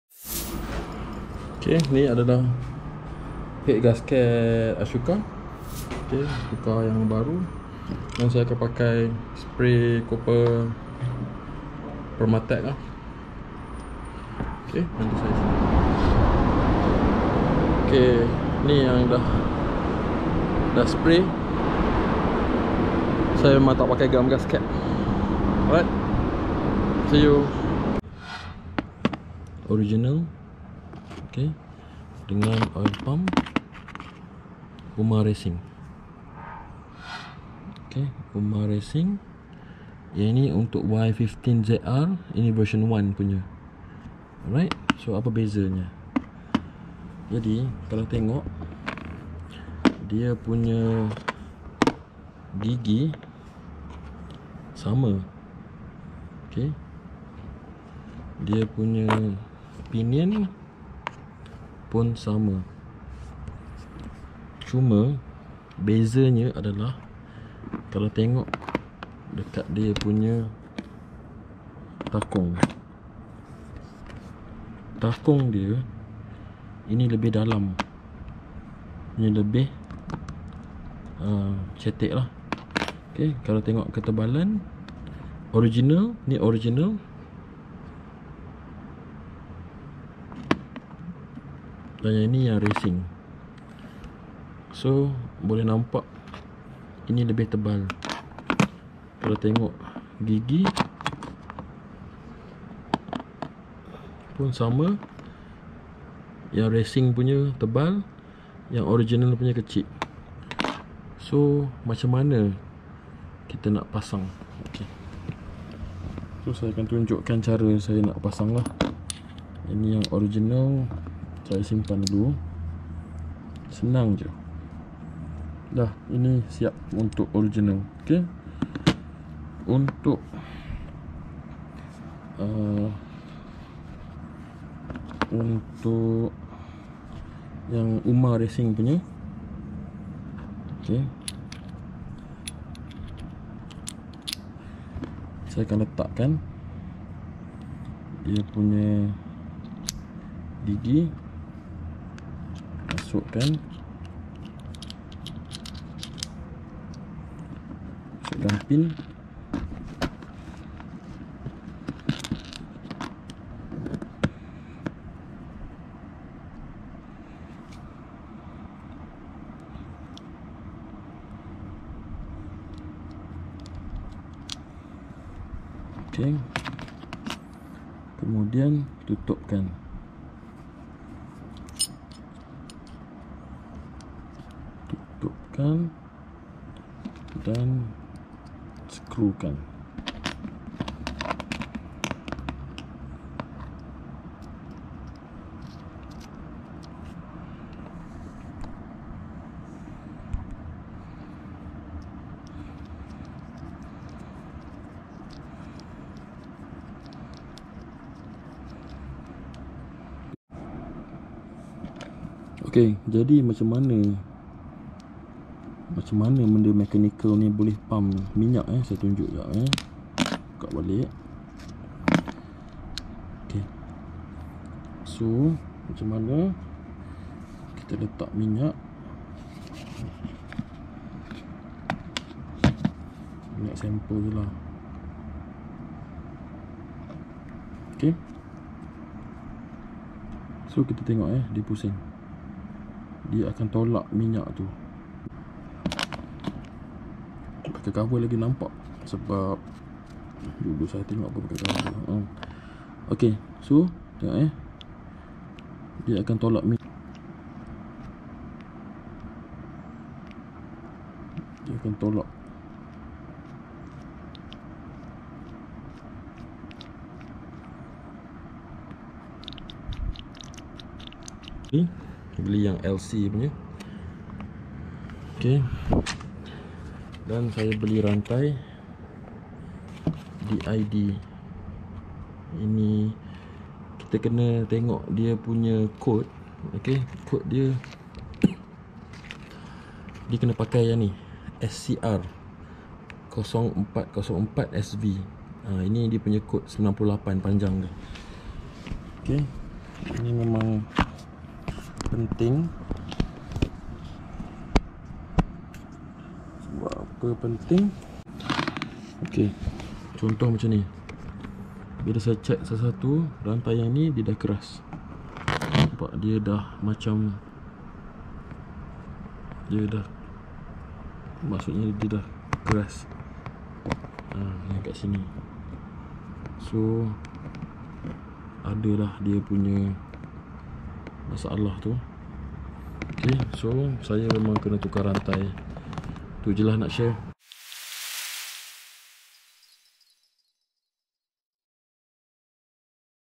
Ep: 27 Part 1 | NVX Engine Rebuild | GASKET PREP, BEZA & CARA KERJA OIL PUMP RACING
PART 1: GASKET PREP,
BEZA & CARA KERJA OIL PUMP,
KENAPA KENA TUKAR TIMING CHAIN,
ADJUSTABLE CAM GEAR